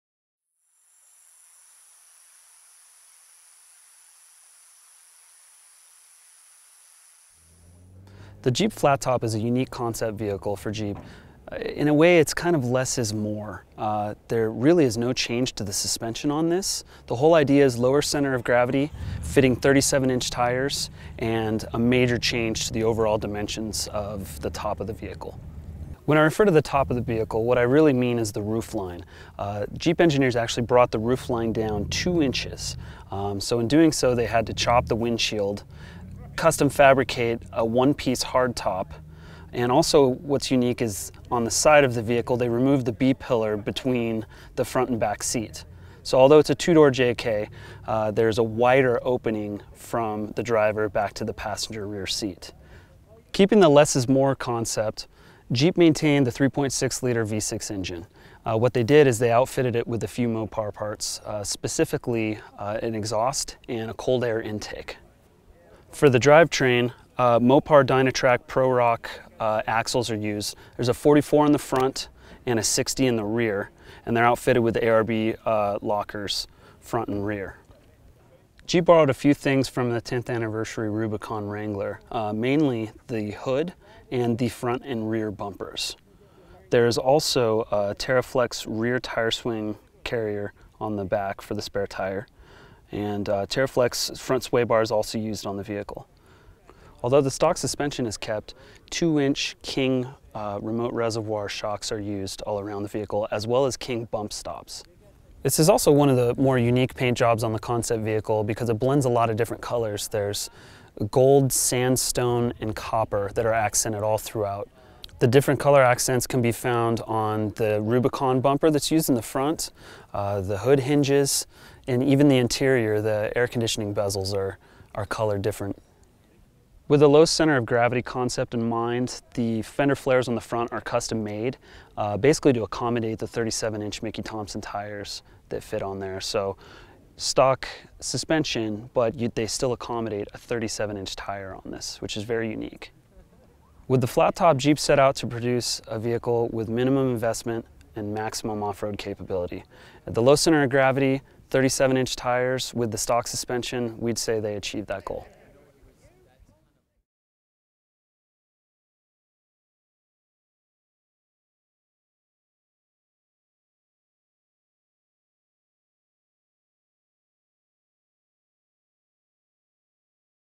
Jeep Flattop Concept - 2013 Easter Jeep Safari
Going for an upscale look, Jeep unveiled the Wrangler Flat Top Concept for the 2013 Easter Jeep Safari.